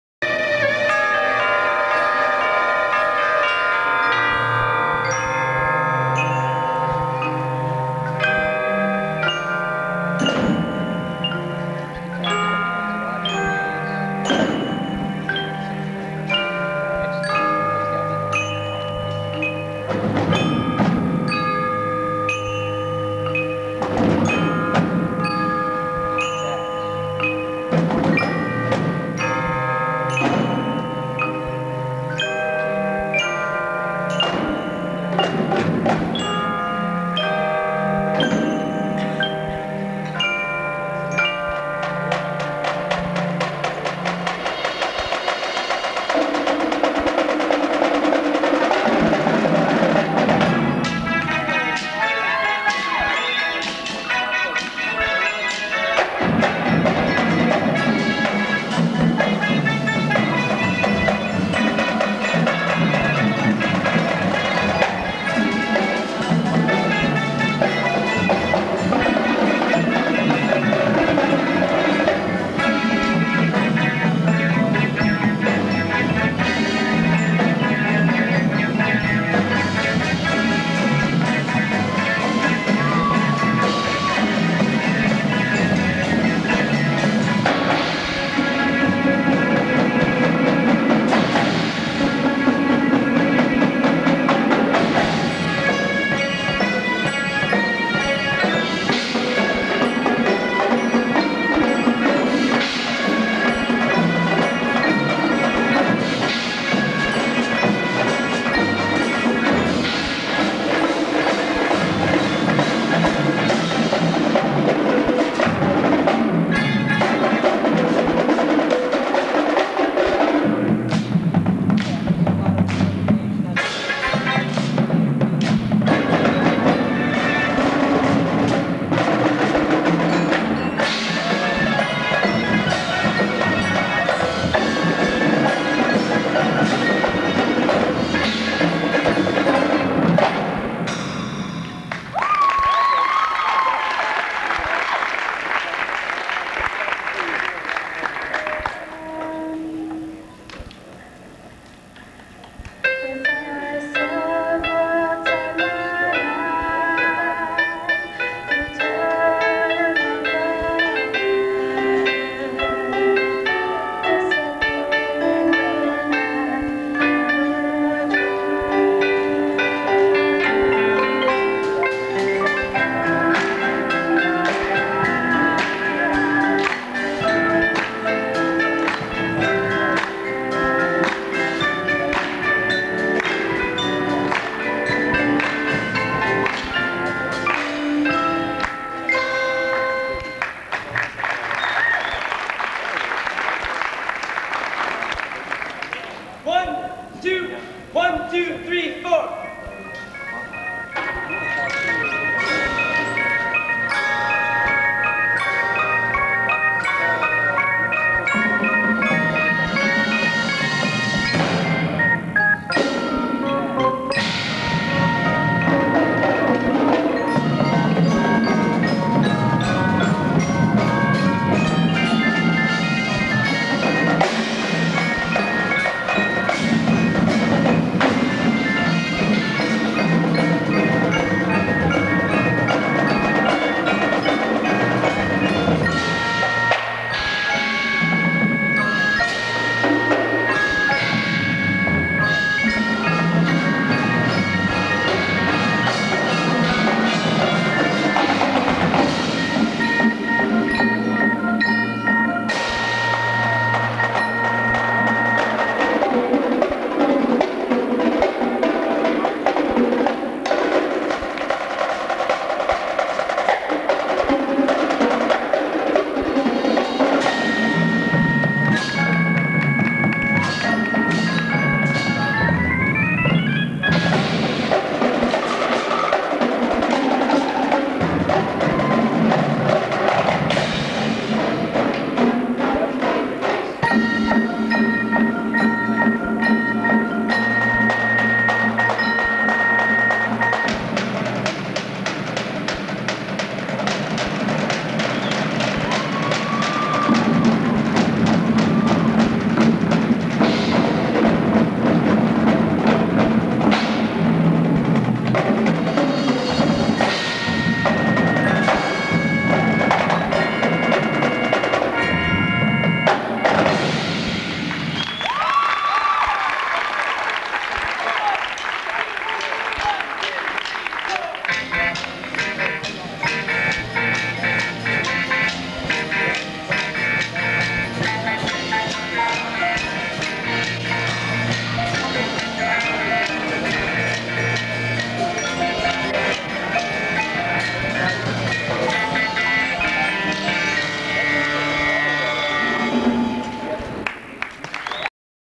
1999 GCHS WDL - Boston
This is the Grant Community High School Winter Drumline performing music of the rock band "BOSTON" at a Drum Contest at Lake Park High School in March of 1999.